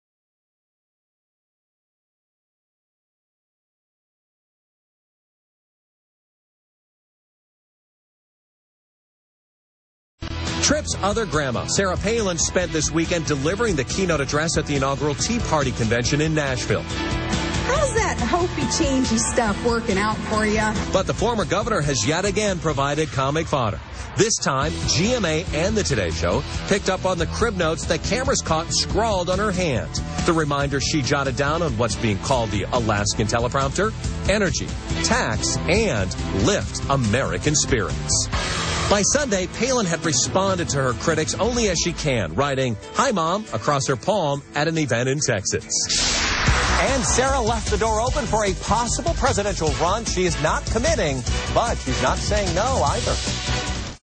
Sarah 9 Palin hand notes 2-6-2010
THIS VIDEO WENT VIRAL BECAUSE OF --- Sarah Palin pathtically reading chat notes scrawled on hr hand at the Tea Party Convention Q...